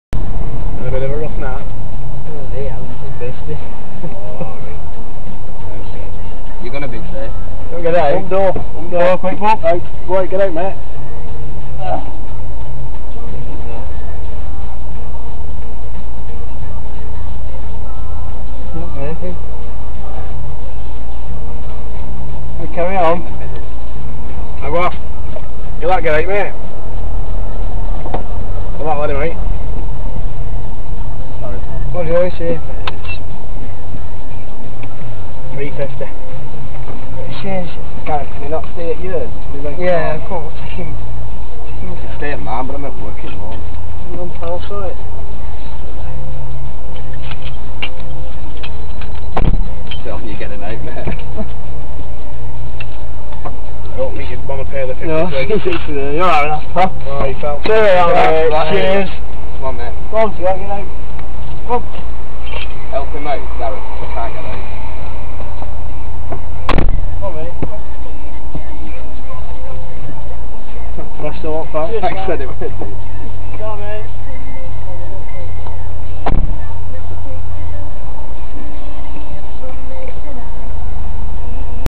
He's gonna be sick...
Journey gets cut short due to lad needing to spew!